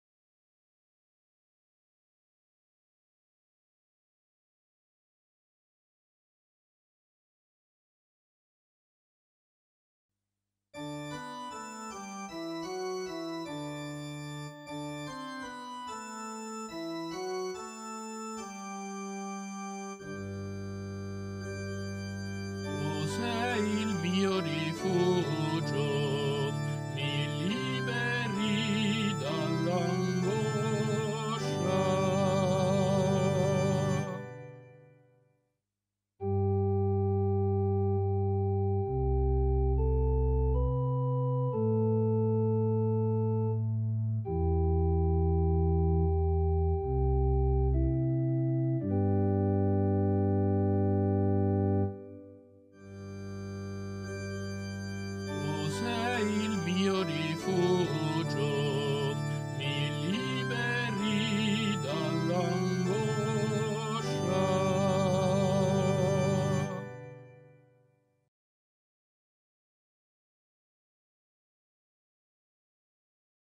VI Domenica del Tempo Ordinario | anno B
Tu sei il mio rifugio, mi liberi dall’angoscia.

Beato l’uomo a cui è tolta la colpa
e coperto il peccato.
Beato l’uomo a cui Dio non imputa il delitto
e nel cui spirito non è inganno.

Ti ho fatto conoscere il mio peccato,
non ho coperto la mia colpa.
Ho detto: «Confesserò al Signore le mie iniquità»
e tu hai tolto la mia colpa e il mio peccato.

Rallegratevi nel Signore ed esultate, o giusti!
Voi tutti, retti di cuore, gridate di gioia!